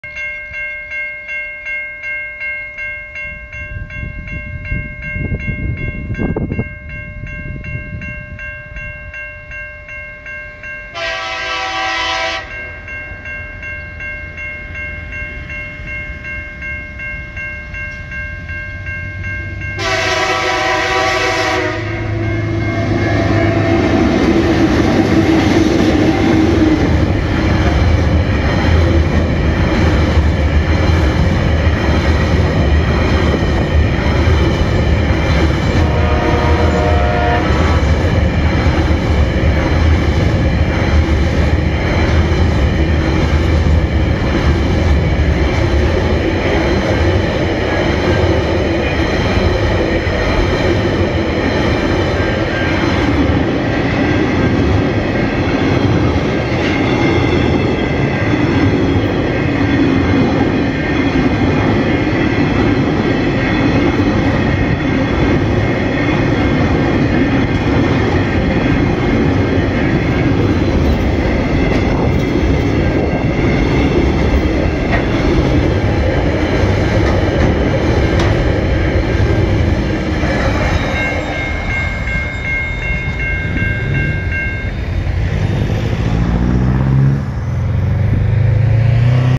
TasRail TR03 TR05 #33 Empty Coal Container Empty Log train crossing Johnston Road
TasRail #433 train lead by TR03 crossing Johnston Road heading to Western Junction with the THGY Coal wagons in the consist.

21:01 Wednesday the 12th of February 2020.